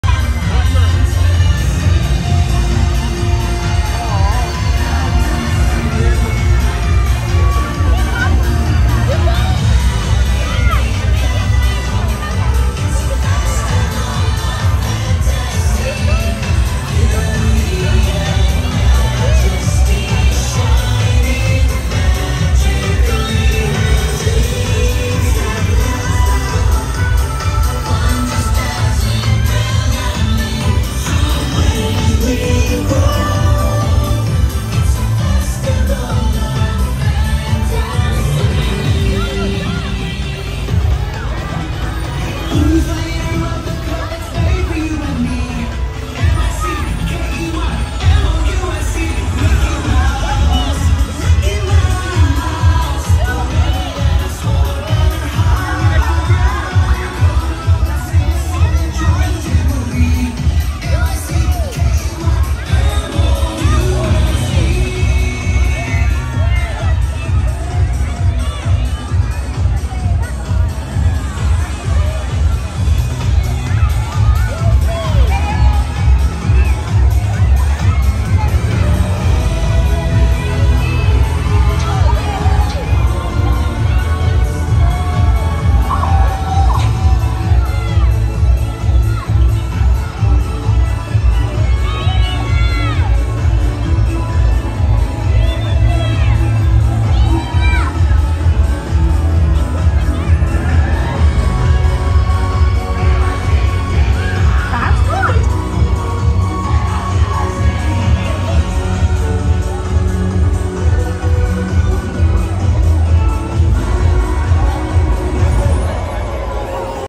Festival of Fantasy Parade
Festival of Fantasy Parade is a must see for children because they can see all the characters in it. The best spot to see it is on Main Street and find a spot one hour before the parade starts. While you film the parade, you can take photos at the same time while you video it and the images will not be blurry in your photos. I have the same name magicparkmemories on tik tok and Instagram and YouTube. I appreciate everyone following me.